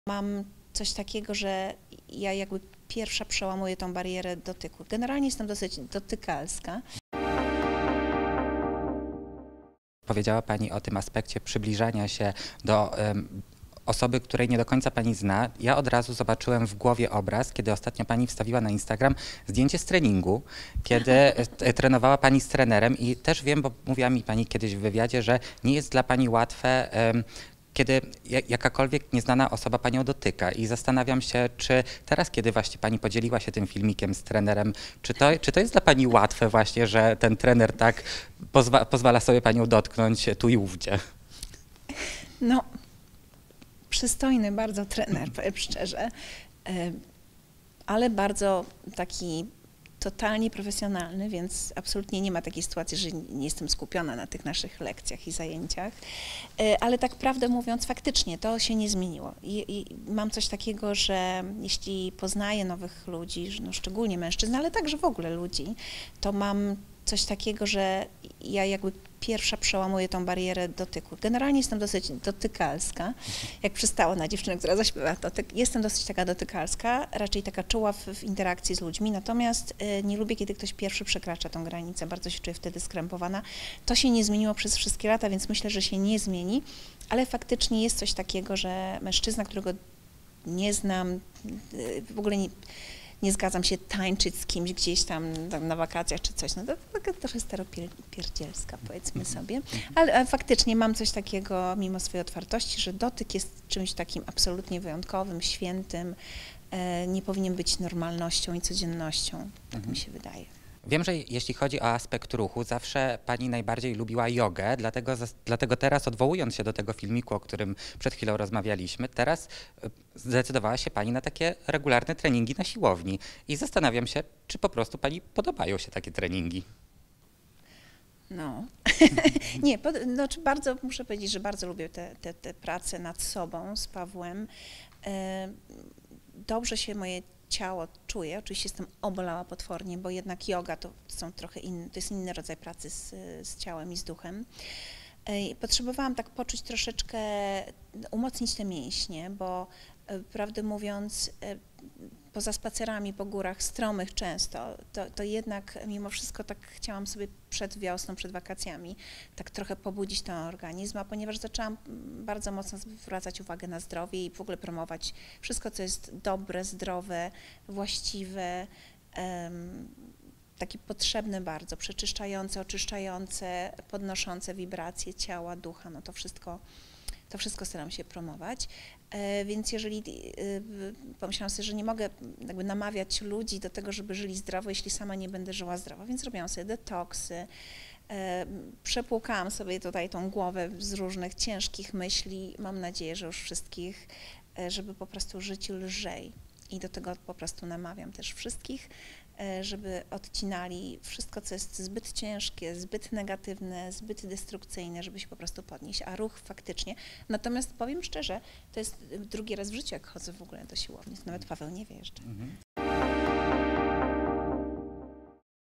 Nowy mężczyzna w życiu Edyty Górniak. „Jest bardzo przystojny...” | FAKT.PL
Edyta Górniak rozpoczęła nową przygodę ze sportem. Piosenkarka regularnie ćwiczy obecnie z trenerem personalnym. Opowiedziała nam o tym. - Mój nowy trener jest bardzo przystojny, ale i również bardzo profesjonalny, dlatego nie ma sytuacji, że nie jestem skupiona na naszych lekcjach i zajęciach. Ale mam tak, że jeśli poznaję nowych ludzi, to ja zawsze przełamuję barierę dotyku. Jestem czuła w interakcjach, ale nie lubię kiedy ktoś pierwszy przekracza tę granicę. Czuję się skrępowana. Mimo swojej otwartości uważam, że dotyk jest święty, wyjątkowy - mówi nam Edyta Górniak.